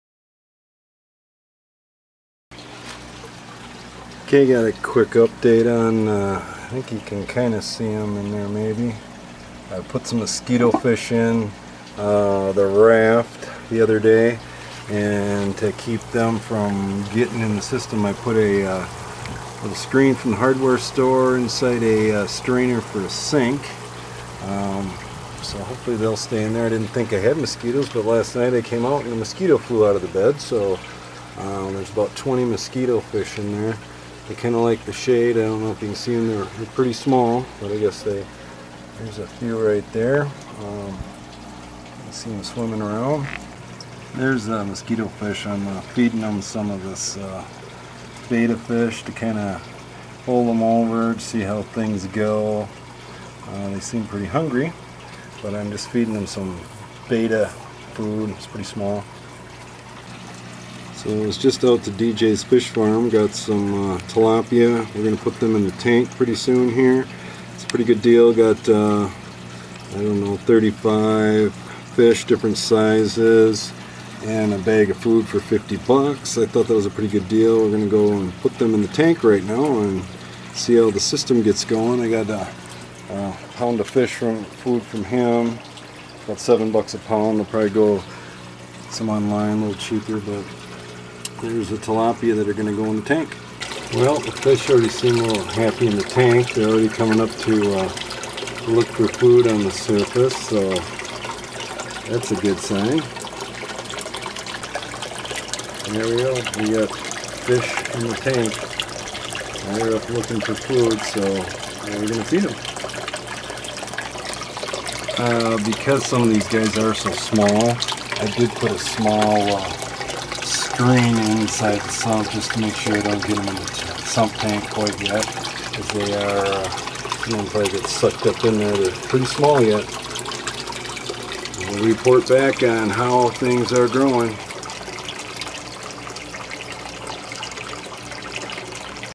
Tilapia Added To Fish Tank & Mosquito Fish Added To Raft Bed of Aquaponics Systems
After a couple of weeks since starting up his Habitat 32 aquaponics system, Jon has added tilapia to his fish tank! He also added some mosquito fish to the floating raft bed since he had noticed some mosquitos. We will add updates as we get them of Jon's system in Phoenix. See the start up and following along!